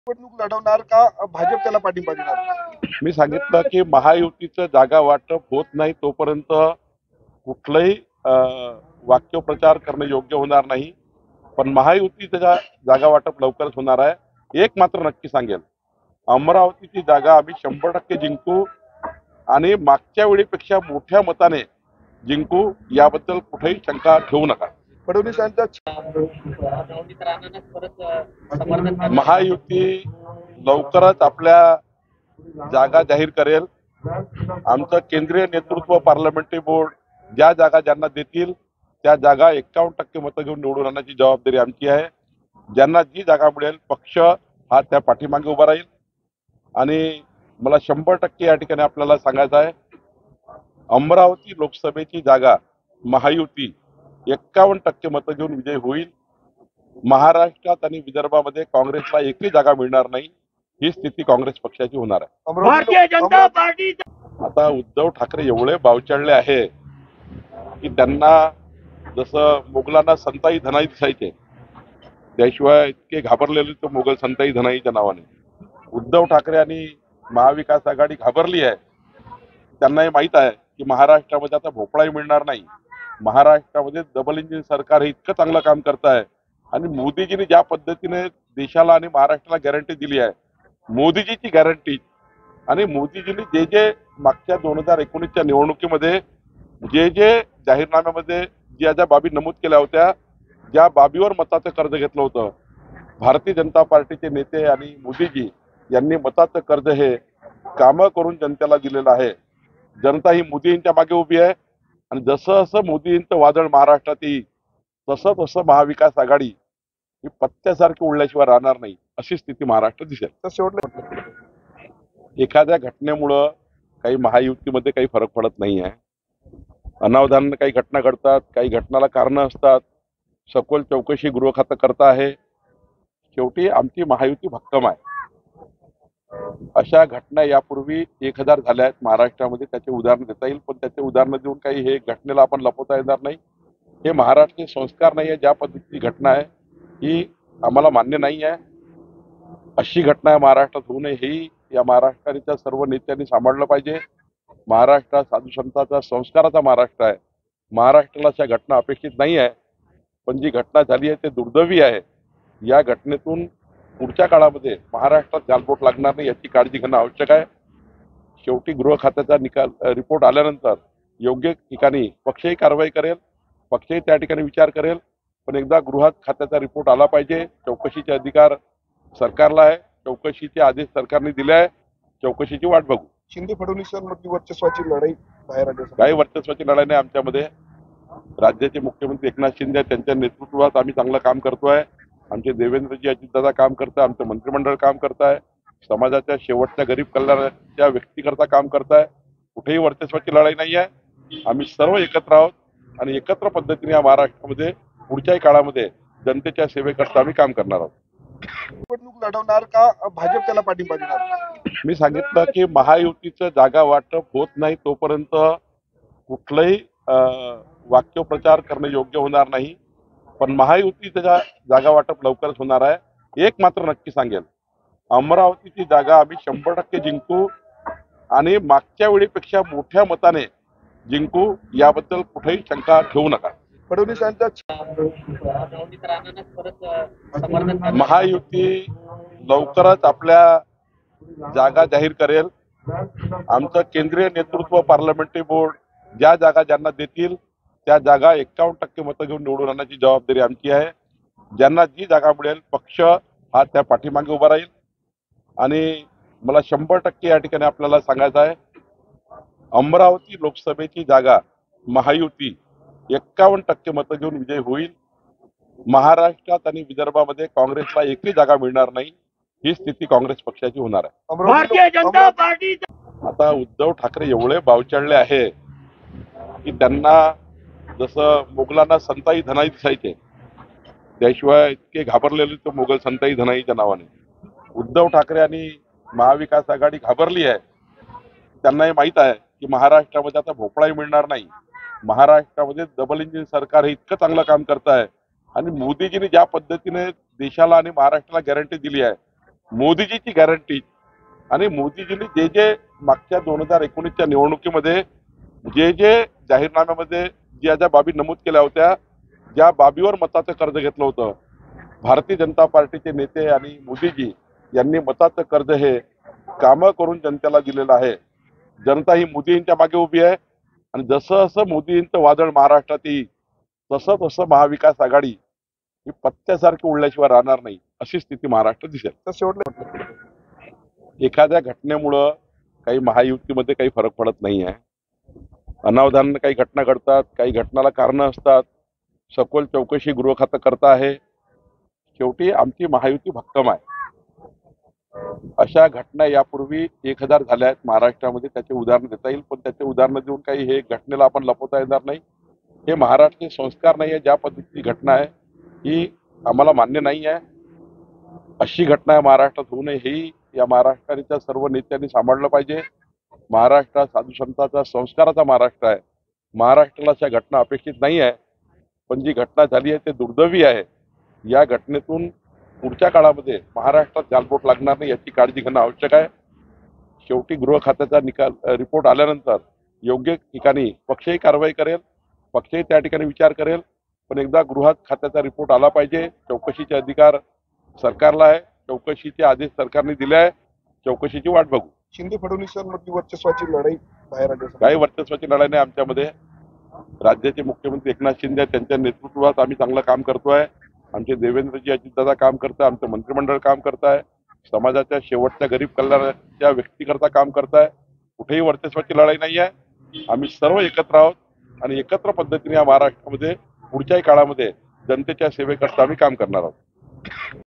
AMRAVATI | एखाद्या घटनेमुळे महायुतीत फरक पडत नाही आहे - चंद्रशेखर बावनकुळे
ON उल्हासनगर गोळीबार
- एखाद्या घटनेमुळे महायुतीत फरक पडत नाही.
- घटनेची सखोल चौकशी गृहखात करते आहे.
- अशा घटना यापूर्वी 1000 वेळा घटल्या आहेत. पण हे संस्कार मान्य नाही.
- झालेली घटना दुर्दैवी आहे, पण ही घटना लपवता येणार नाही.

ON उद्धव ठाकरे
- उद्धव ठाकरेंसह माविआ घाबरली आहे. तर माविआला आता भोपळा पण मिळणार नाही.
- माविआ आता पत्यासारखी उडणार आहे.

ON लोकसभा जागावाटप
- महायुती लवकरच आपल्या जागा जाहीर करेल.
- तर 51 टक्के जागा मिळणार असून अमरावती लोकसभाची जागा महायुतीचाच उमेदवार 51 टक्के घेऊन जिंकणार तर विदर्भात एकही जागा काँग्रेसला मिळणार नाही.
- मागच्या वेळी पेक्षा जास्त फरकाने अमरावतीची जागा जिंकू.

BYTE - चंद्रशेखर बावनकुळे, भाजप प्रदेशाध्यक्ष